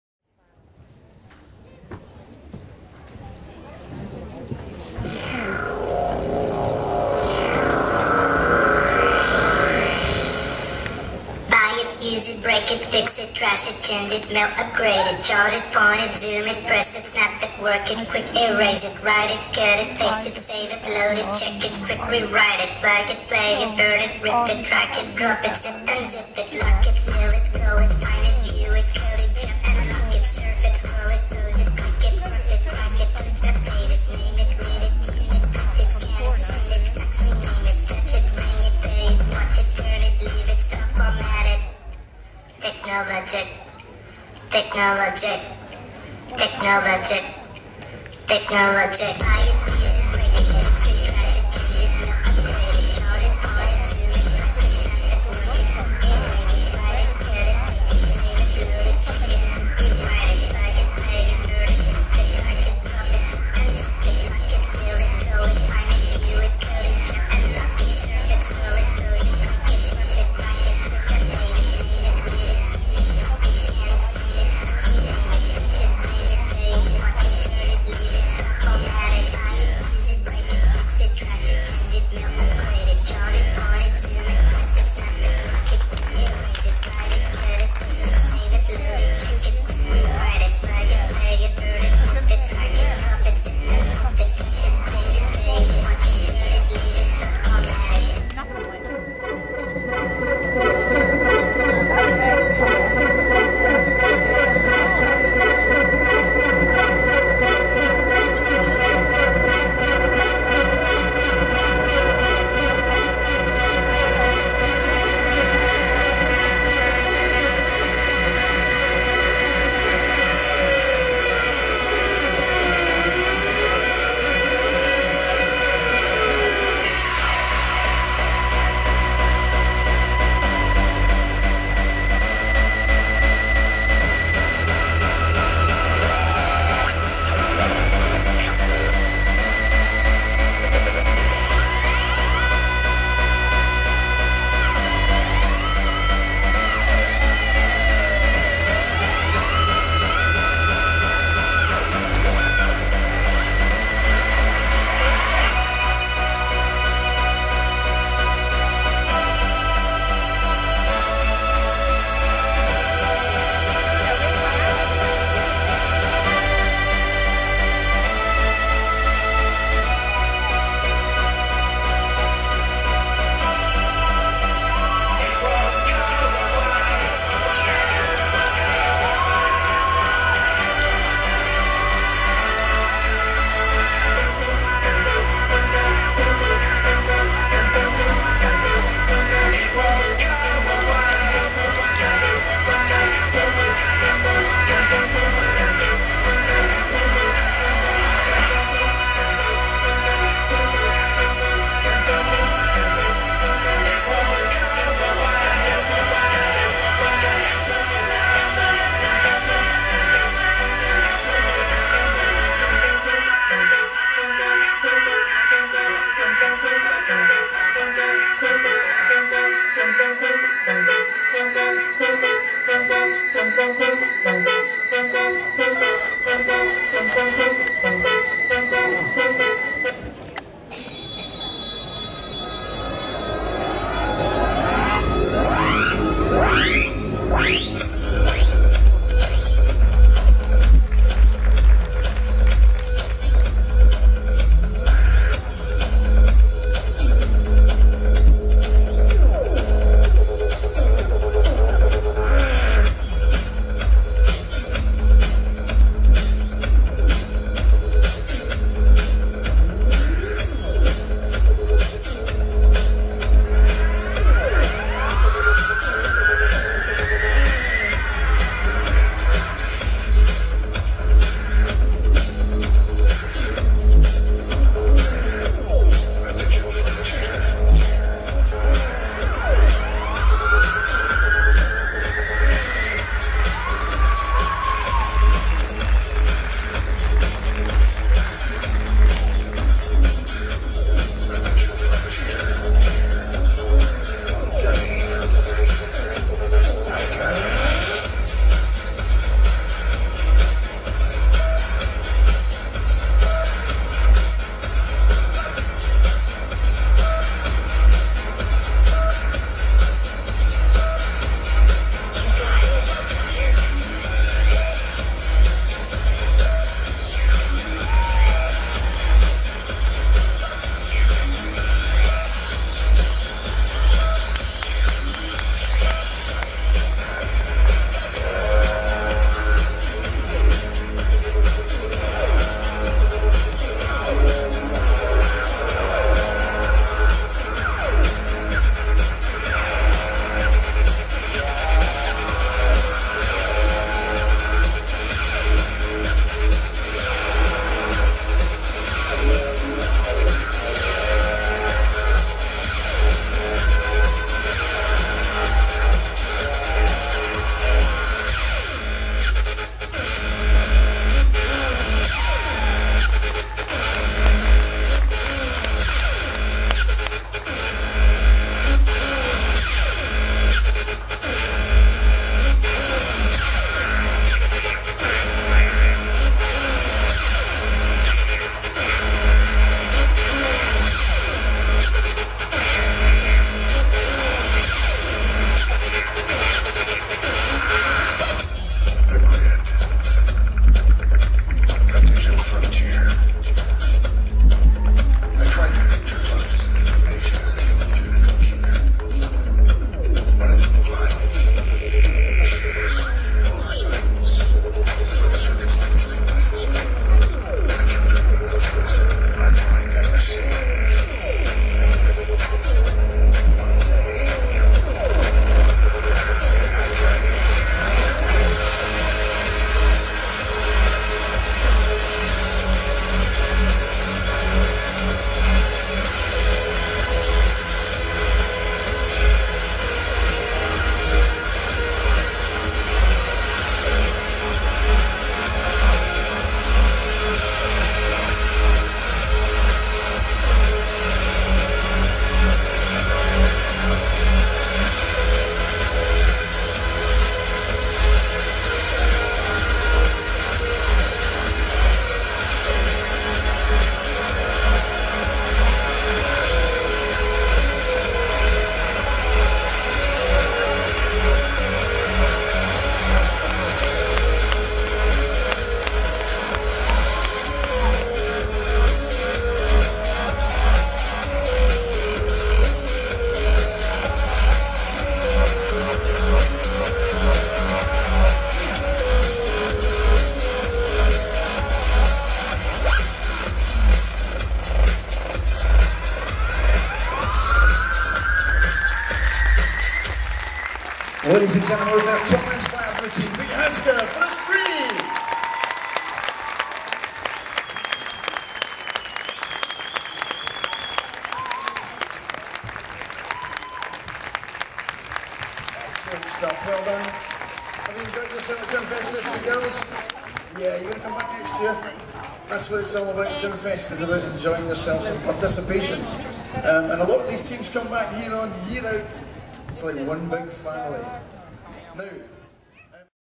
Esprit_Tron_Gymfest_Irvine_060512.wmv
Swindon Esprit Gymnastics Display Team.
Tron display in The Magnum Centre, Irvine, near Glasgow, Scotland for the British Gymfest in May 2012